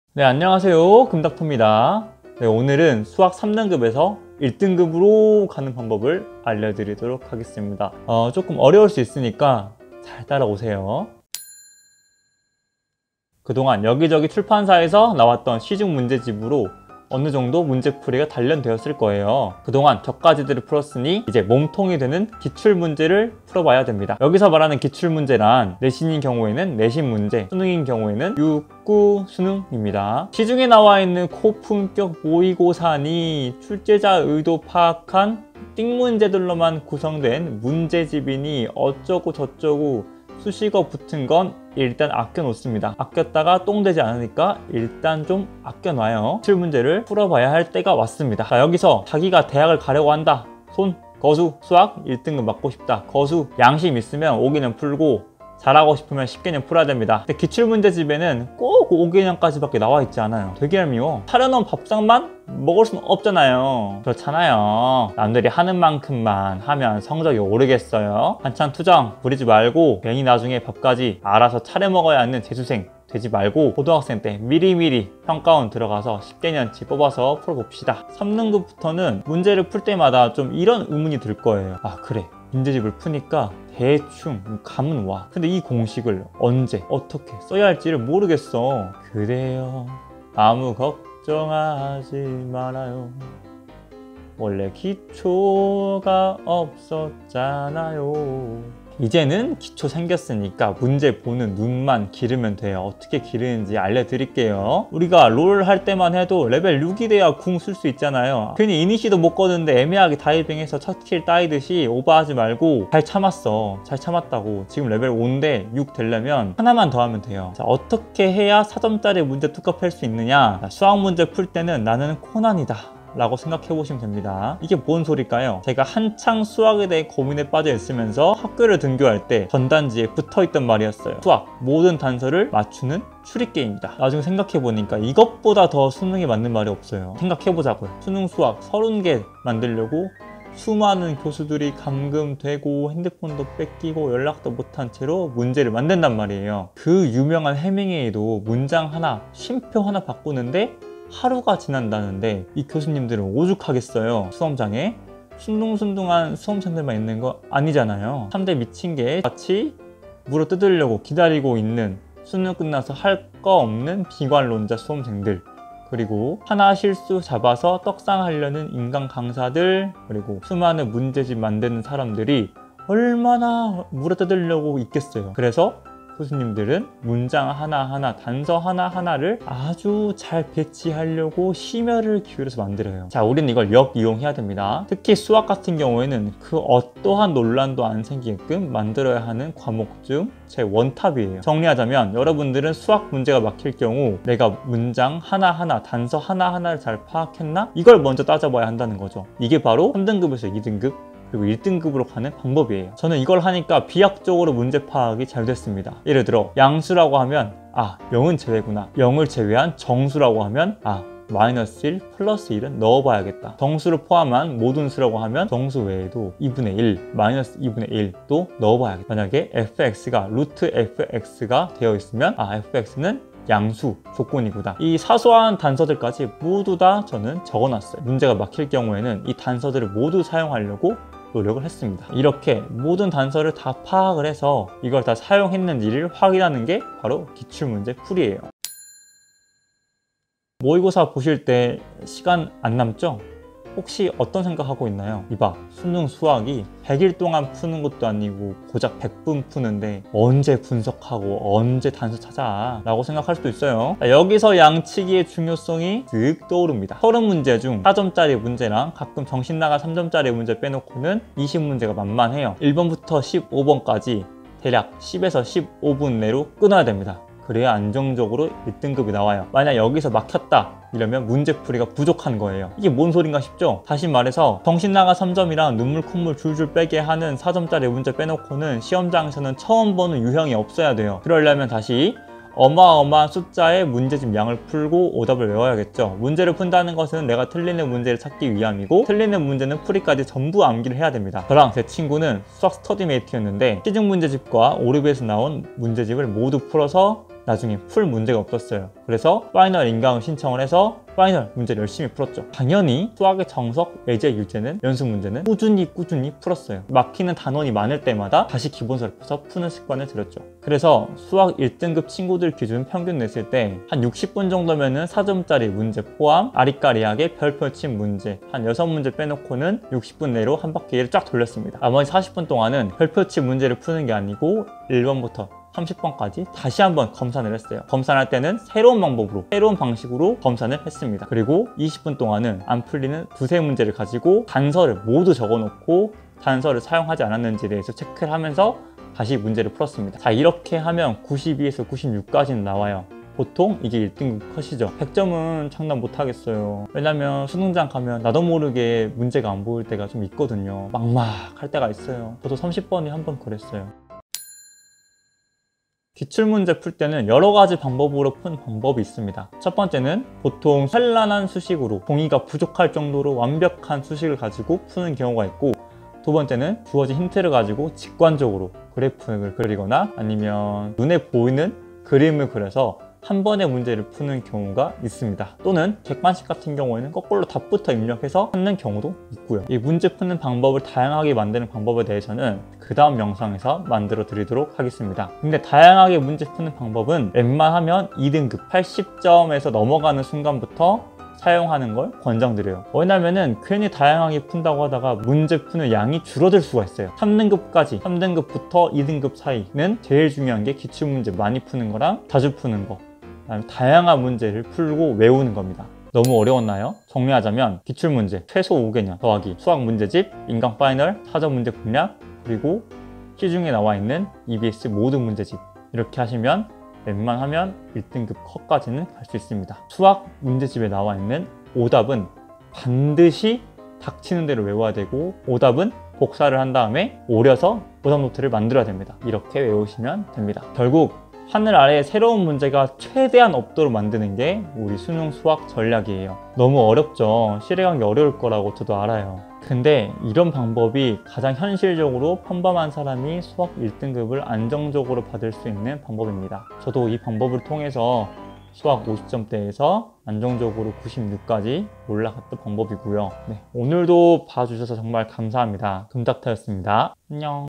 [의대생 공부법] 수학 3등급에서 1등급으로가는 공부법 - 수학 공부법 2탄
저번에 알려드렸던 수학 5등급에서 3등급편을 이은
2탄입니다.

저번 영상보다는 다소 어려울수도 있지만, 최대한 이해가 되시도록 영상을 준비했습니다 .

정리하자면,
결국 양과 질입니다.